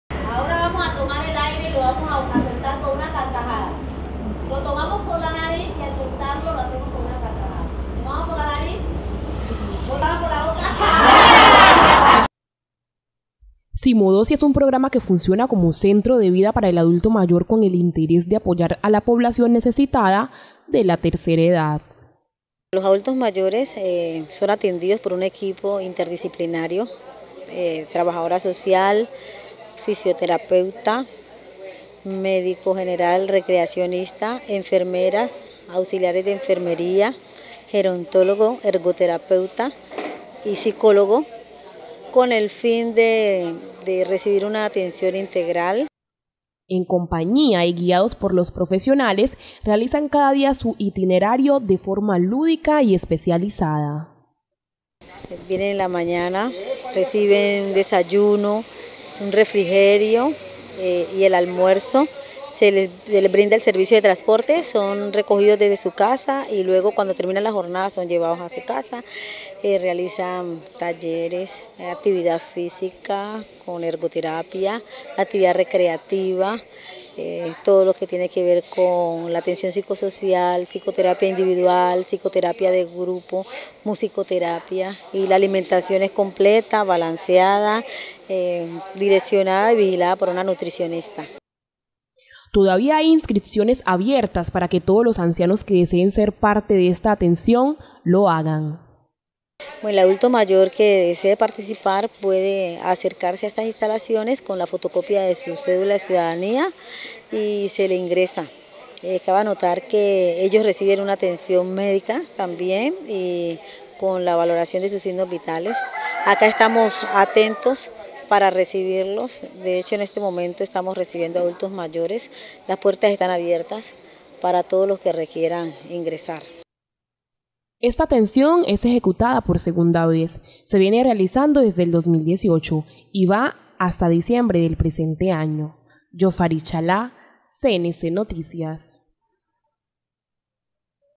Ancianos reciben atención en salud y recreación
Atención médica, psicológica, pediatrica, lúdica, entre otras, son las que reciben adultos mayores de quibdó en el programa simodocia que funciona como centro de vida para esta población